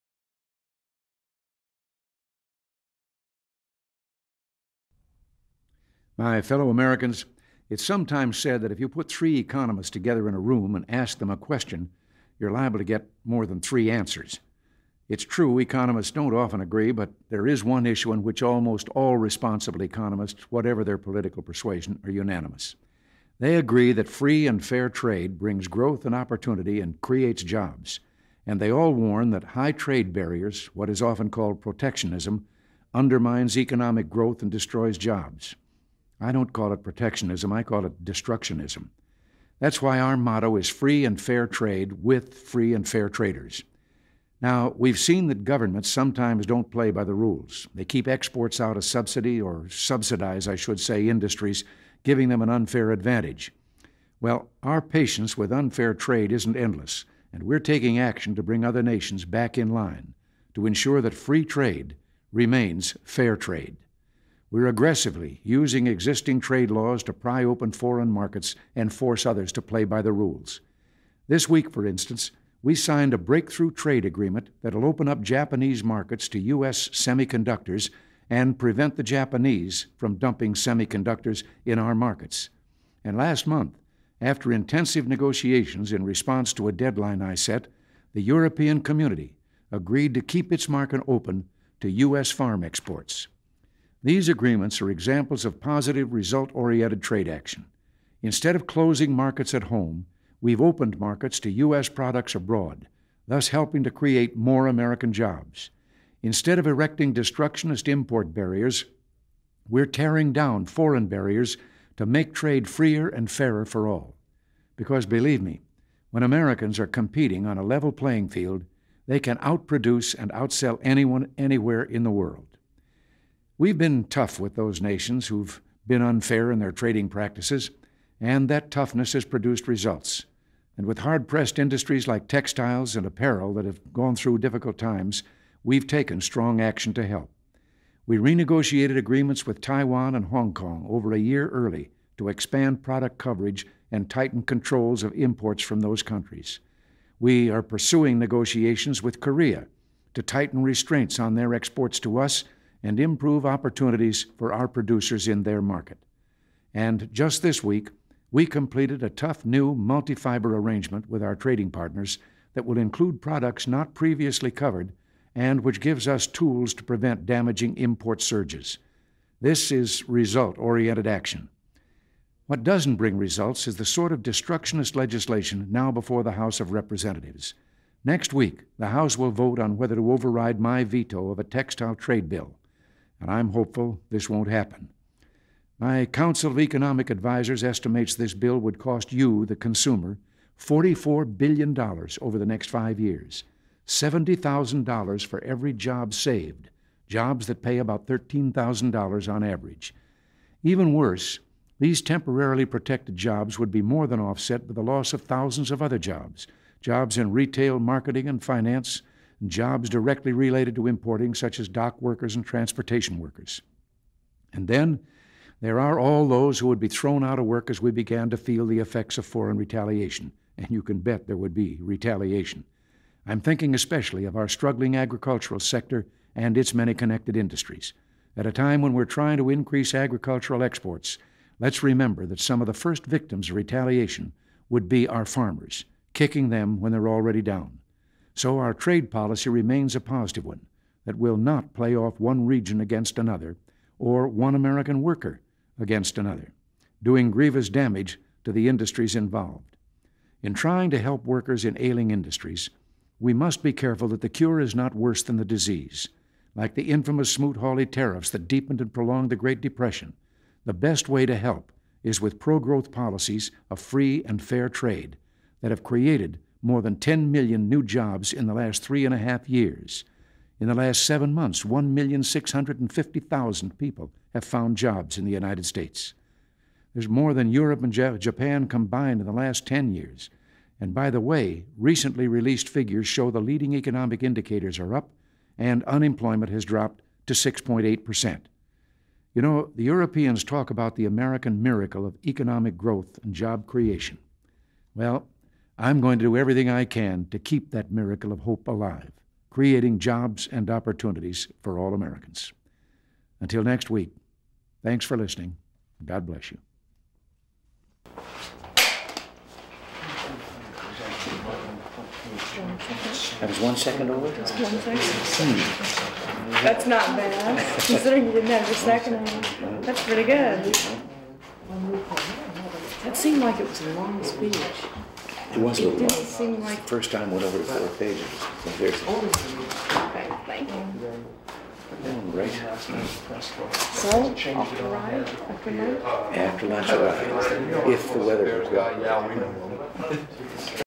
President Reagan's Radio Address on Free and Fair Trade on August 2, 1986
Full Title: President Reagan's Radio Address on Free and Fair Trade at Camp David, Maryland on August 2, 1986

Access Restriction(s):Unrestricted
Use Restriction(s):Unrestricted